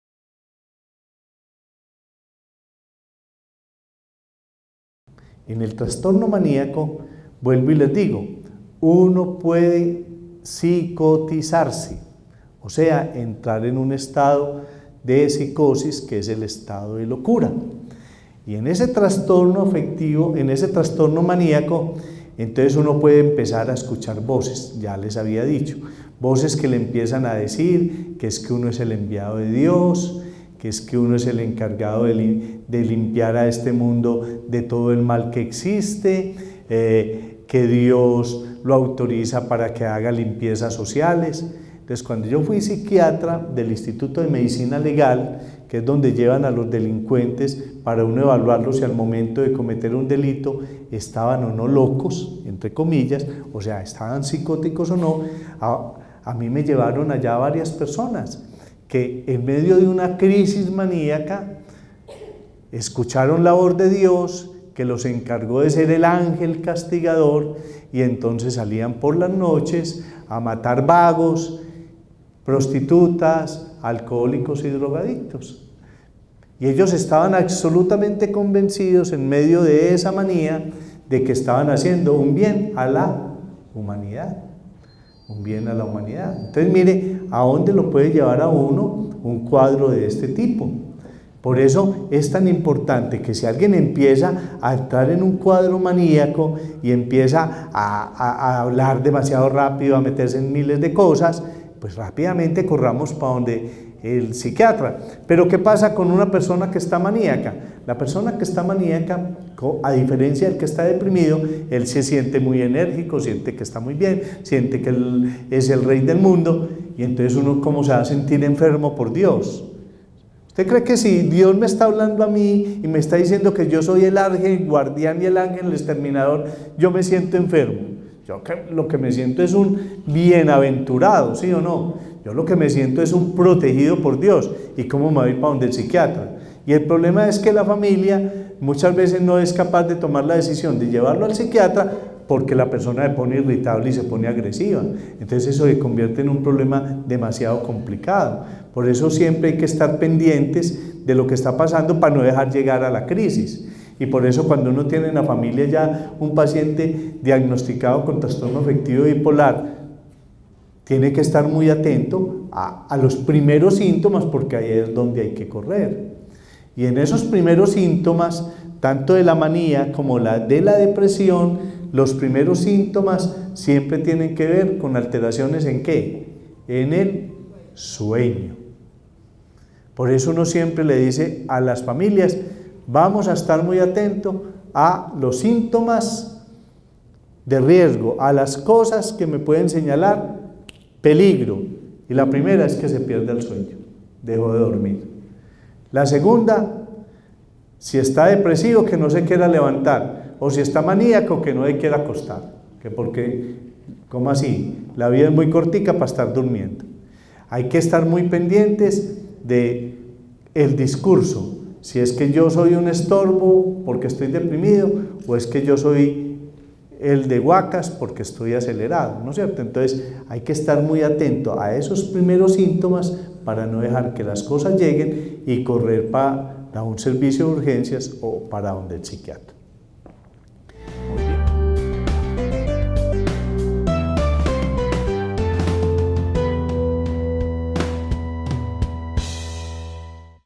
Trastorno Maníaco y Psicosis
Trastorno Maníaco y Psicosis. En el trastorno maníaco se puede entrar en un estado de psicosis, se pueden comenzar a escuchar voces con ideas delirantes, entrar en un estado de locura. Por esta razón es muy importante visitar el psiquiatra cuando se comienza un cuadro maníaco.
Un víde de Ramón Acevedo Cardona, Psiquiatra, Concejal de Medellín.
Presentador del Programa Sanamente de Televid.